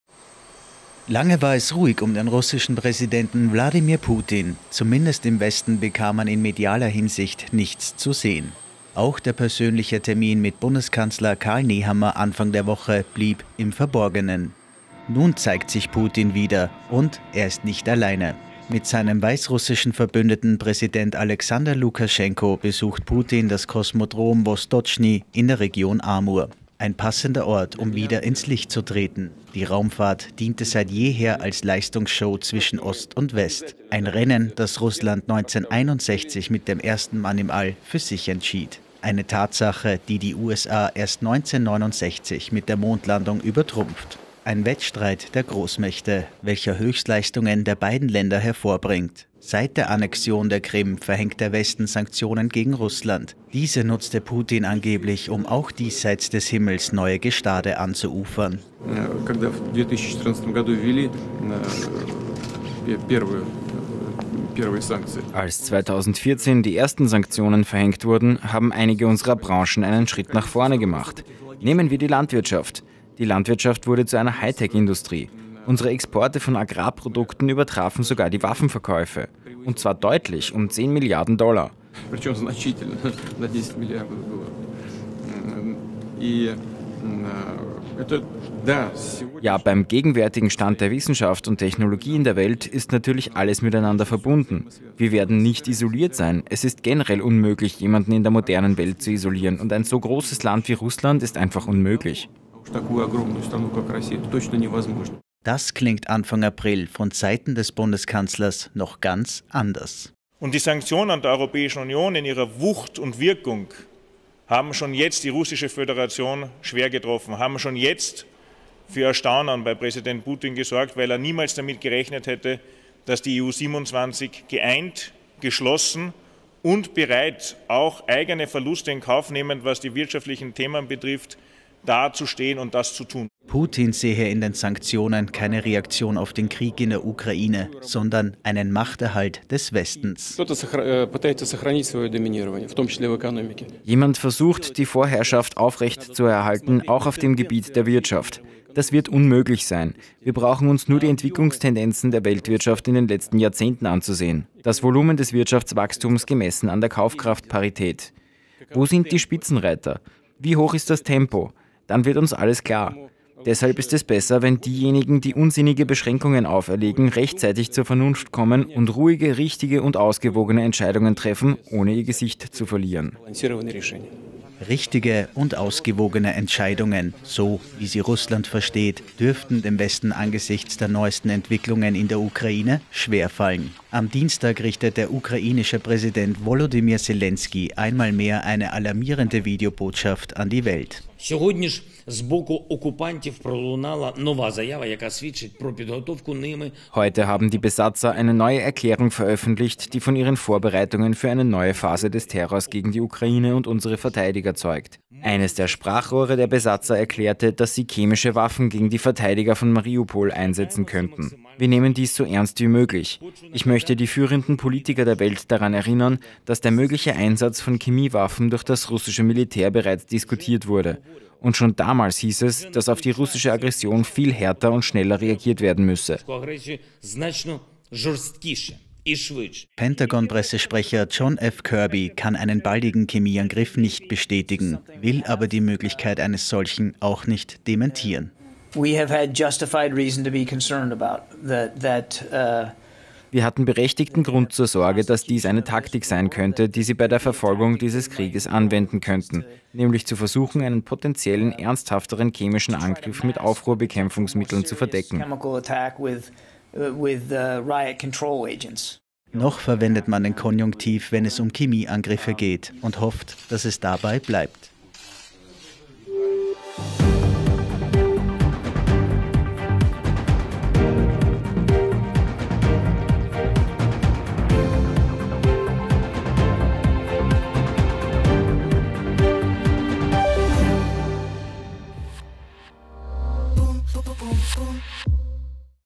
Nach Abwesenheit: Putin wieder zurück in der Öffentlichkeit
Lange war es ruhig um den russischen Präsidenten Wladimir Putin.
Zumindest im Westen bekam man in medialer Hinsicht nichts zu sehen.
Auch der persönliche Termin mit Bundeskanzler Karl Nehammer Anfang der Woche blieb im Verborgenen.
Nun zeigt sich Putin wieder, und er ist nicht alleine.
Mit seinem weißrussischen Verbündeten, Präsident Alexander Lukaschenko,  besucht Putin das Kosmodrom Wostotschny in der Region Amur.